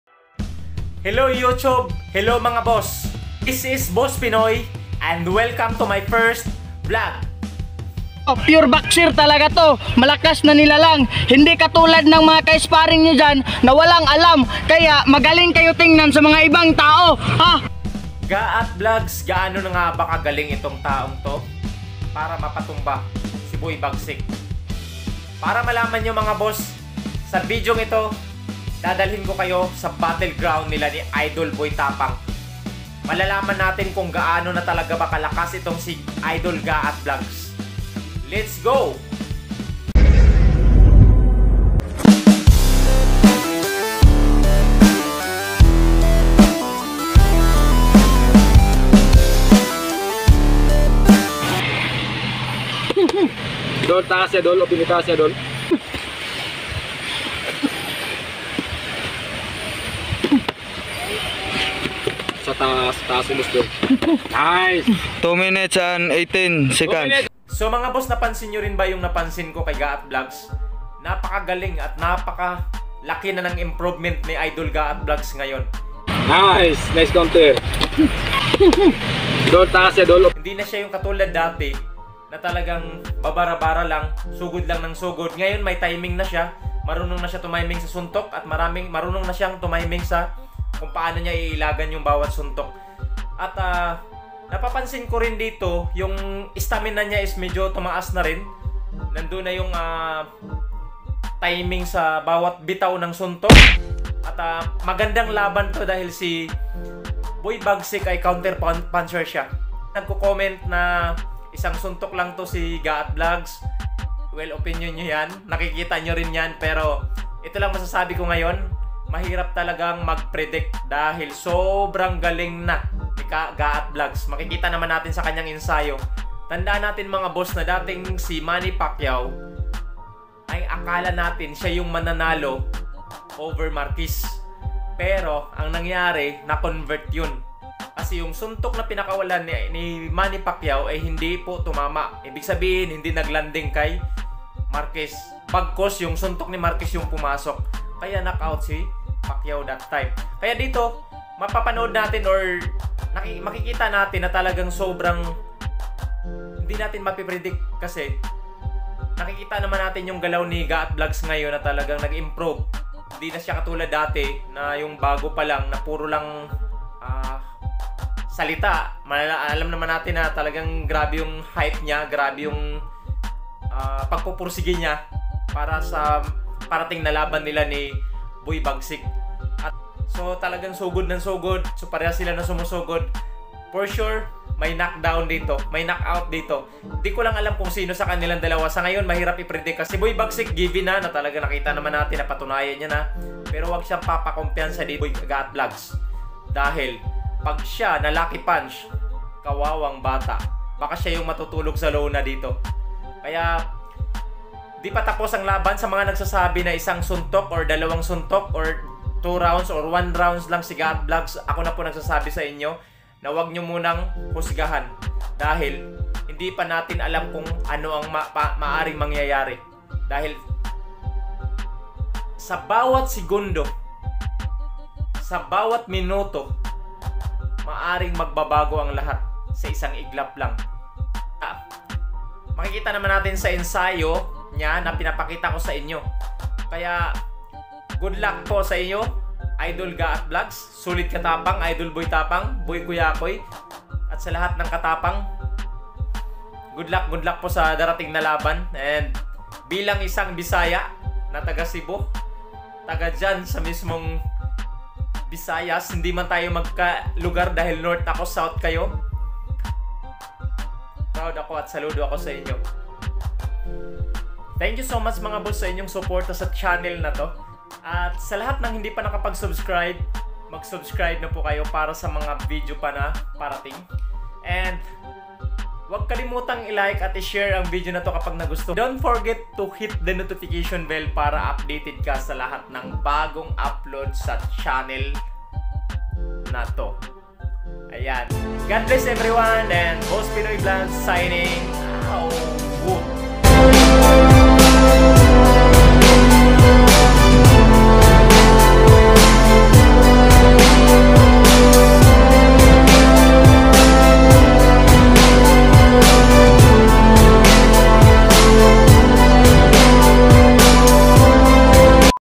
Boy Bagsik supot nga ba Gaat Vlogs?
Boy Tapang Trainor ni Gaat Vlogs💥🔥

Yow wazzup Boss,
In this video mapapanopd ninyo Ang kakaibang Boy Tapang Trainor ni Gaat Vlogs💥🔥.Sobrang laki Ng improvement Dito mga Boss ni Gaat vlogs kaya Isang magandang laban sa Boty3 Ang mapapanood Naten sa darating na laban.Boy tapang trainor ni Gaat Vlogs mga Boss sobrang galing niya mag motivate.